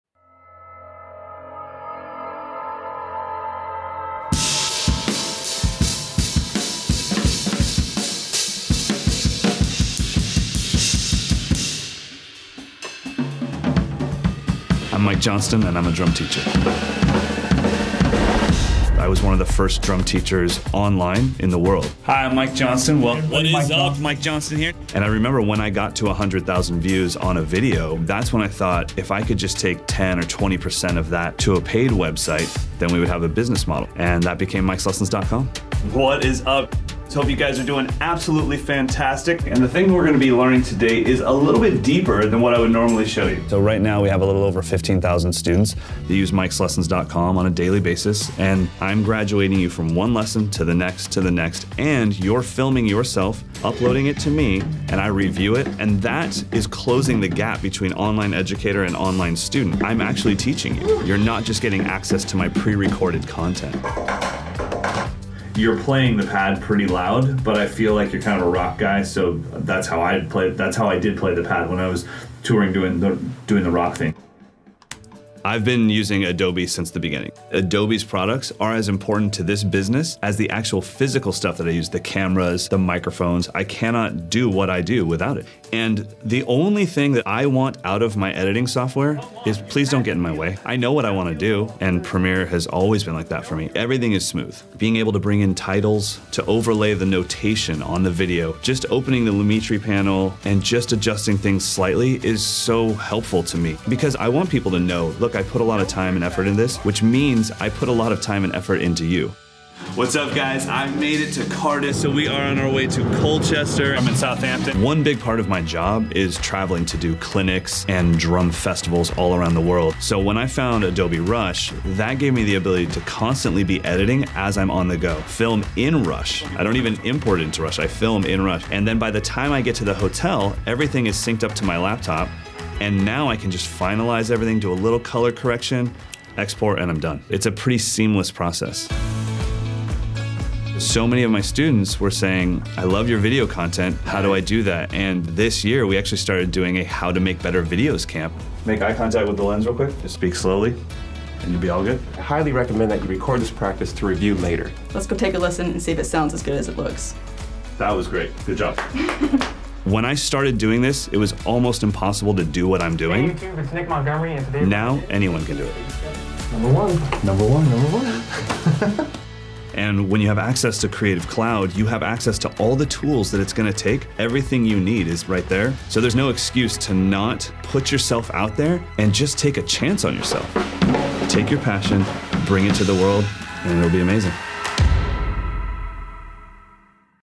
Pioneering Online Instruction with Premiere Pro | Mike Johnston | Adobe Creative Cloud
Learn how MIke Johnston used Adobe Premiere Pro to help lay the foundation for his business as one of the first online drum teachers, creating mikeslessons.com.

Learn more about how Mike used Premiere Pro to launch his online teaching platform with Premiere Pro: